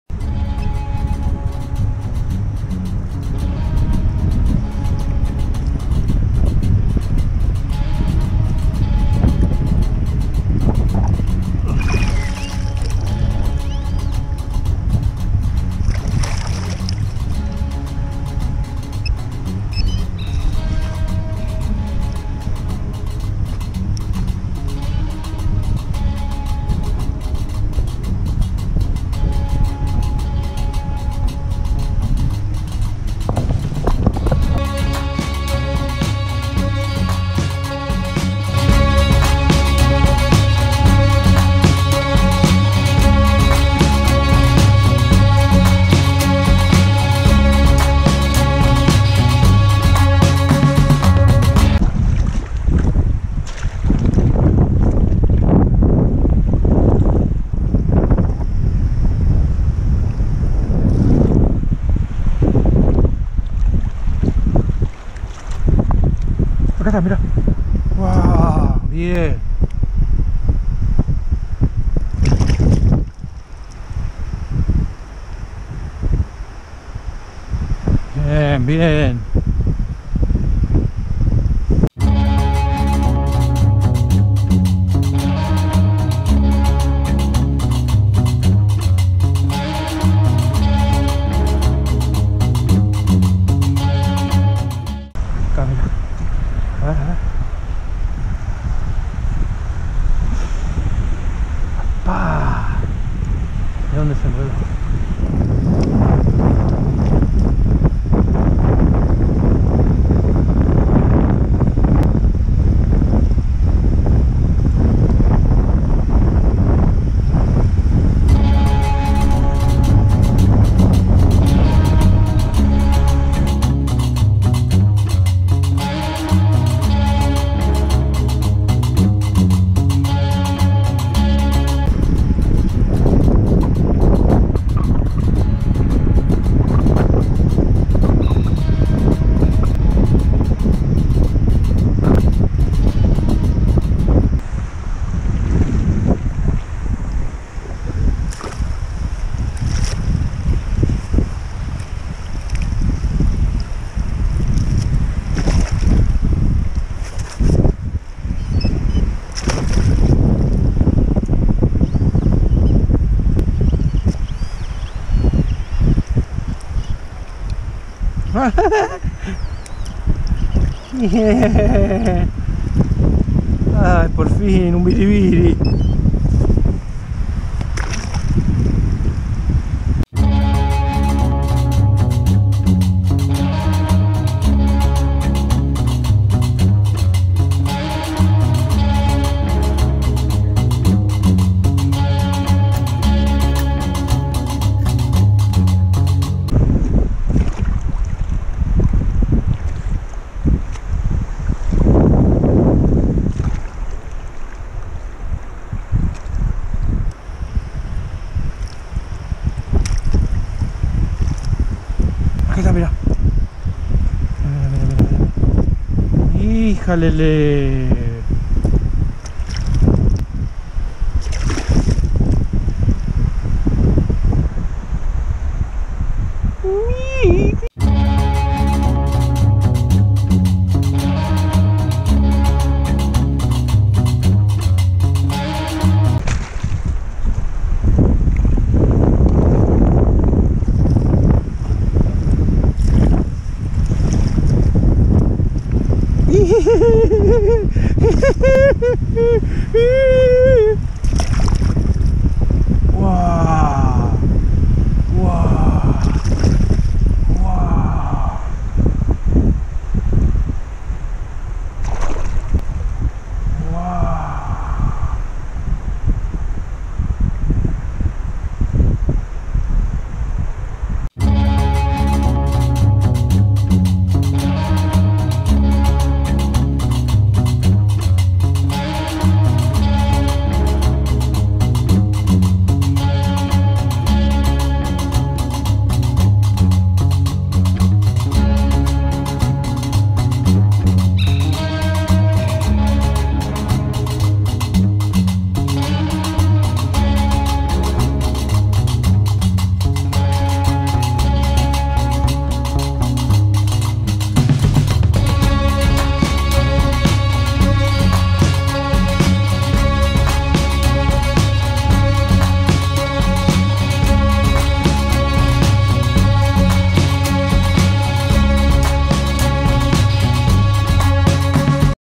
GOLDMAGNET - Detector de oro
Te gustaría ir a la playa y encontrar oro ? A mi es lo que mas me gusta, mira este videito que hice de dos días de búsqueda.